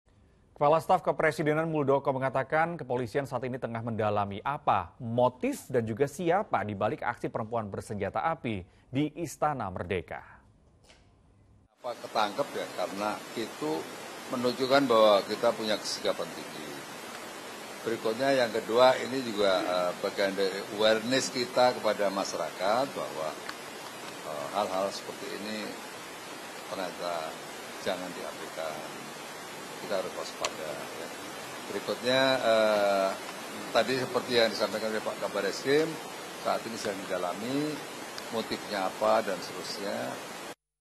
Mengungkap Sosok dan Motif Perempuan Bersenjata Api di Istana Merdeka | Liputan 6
Kepala Staff Kepresidenan, Moeldoko, mengatakan pihak kepolisian saat ini tengah mendalami apa motif dan juga siapa dibalik aksi perempuan bersenjata api di Istana Merdeka.